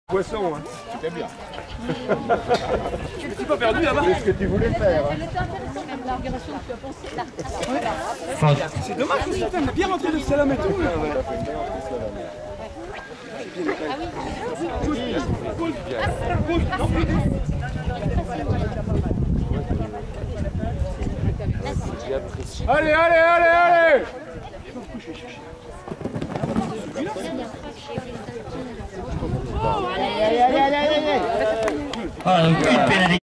Flora jumping cat C plus Messigny Vantoux juin 2014
Concours d'agility Messigny et Vantoux 22 juin 2014, jumping "plus" catégorie C